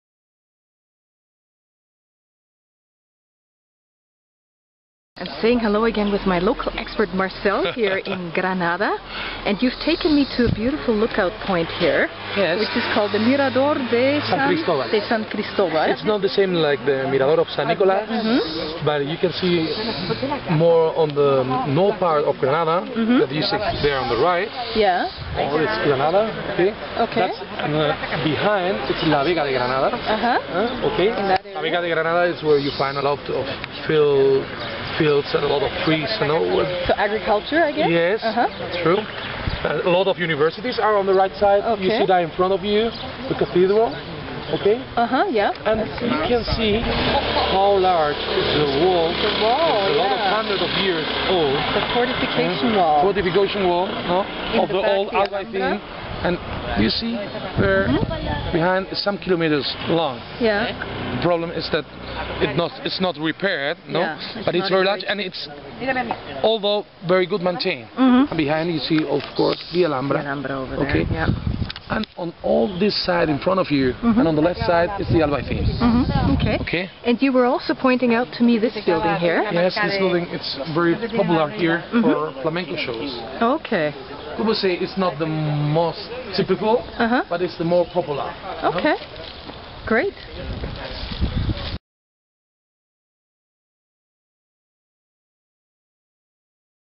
Granada, Andalucia - A Guided Tour by a Local Expert
My second day in Granada (April 12, 2011) was awesome. It started with a morning walk a through the old city centre and a visit to the municipal market. Then I met local expert Marcell Doerrier who is a Granada native and a tourism expert who organizes tours and rents out vacation homes in a small country town outside Granada.

Our city tour started with breakfast on the Plaza Nueva, and continued with a bus ride to the Gypsy Quarter of Sacromonte and a long hike through the narrow streets of Albaycin, the city's Moorish quarter.

Then we continued our expedition downtown to cover the Cathedral, the Alcaiceria (Jewish Quarter), the lively Plaza Bib Rambla and various shopping areas of Granada. Along the way I learned a lot about Andalucia and life in this amazing city. And we got to sample some of the tasty tapas that Granada is so famous for.